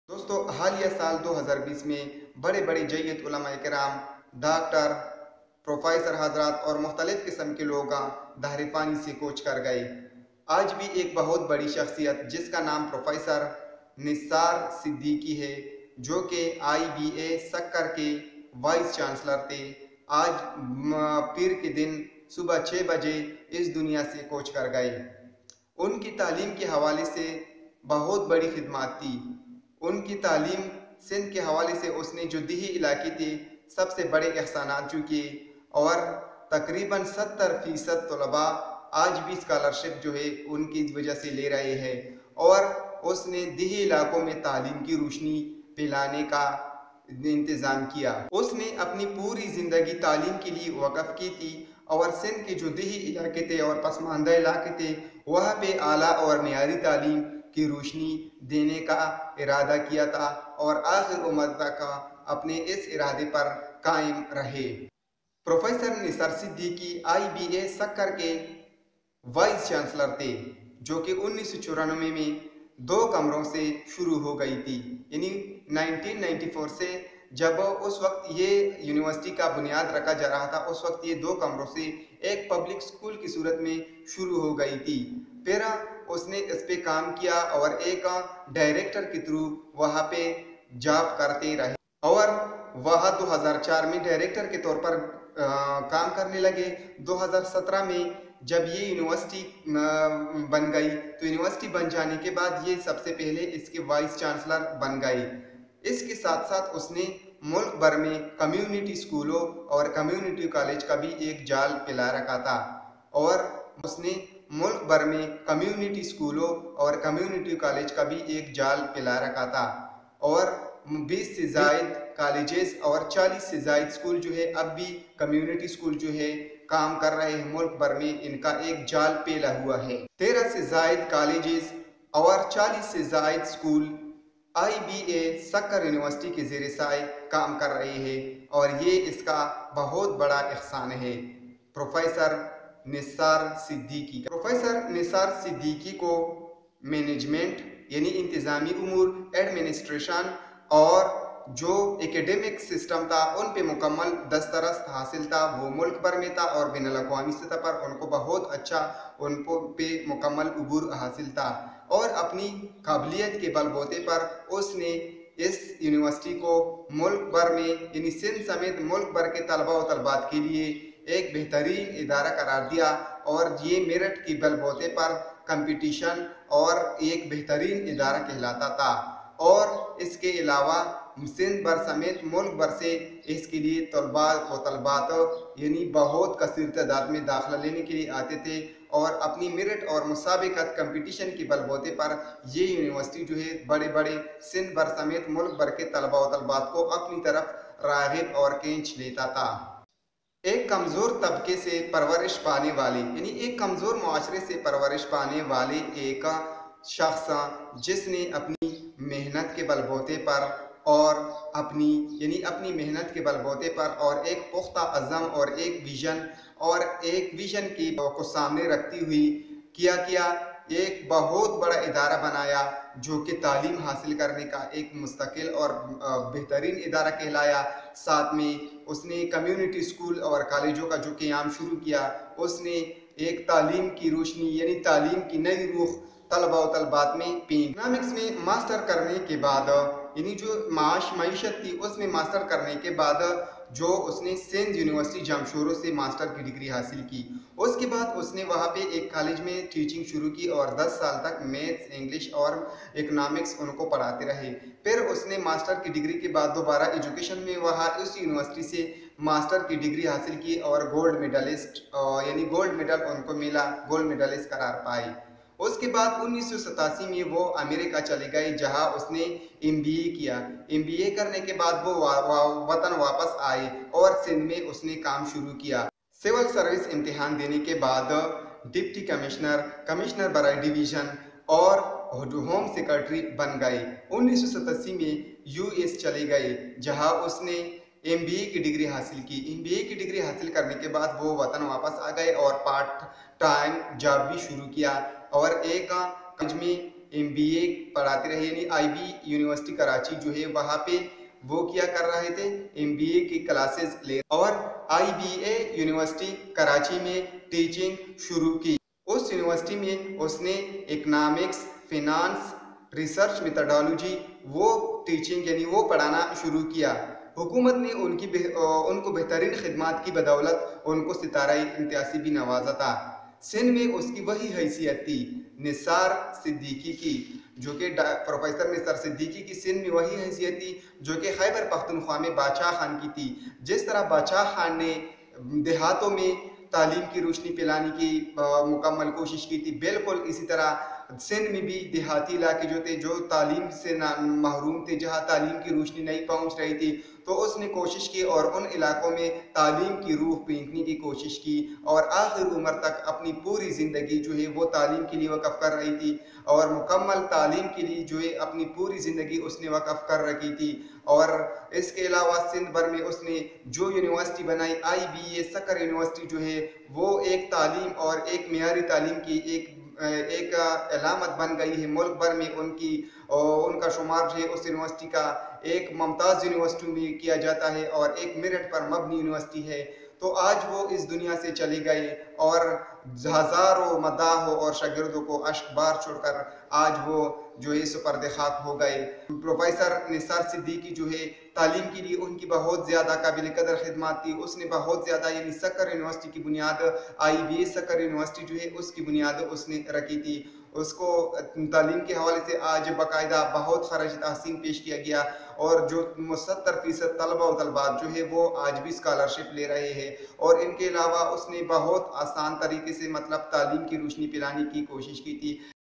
Nisar Ahmed Siddique Life 1944-2020 || Nisar Ahmed Siddique A Great Educational Leader || IBA Sukkar
Famous Educationist Nisar Ahmed Siddique  is no longer with Us.He has spent 76 years.Nisar Ahmed Siddique is known founder of IBA University Sukkar.Due to his tireless efforts IBA sukkur is one the best and top ranking education institution in Pakistan.A Legend departs but his legacy stays on and thrives.
Pioneer Of Education
Community Schools
Community Collages
Nisar Ahmed Siddique Life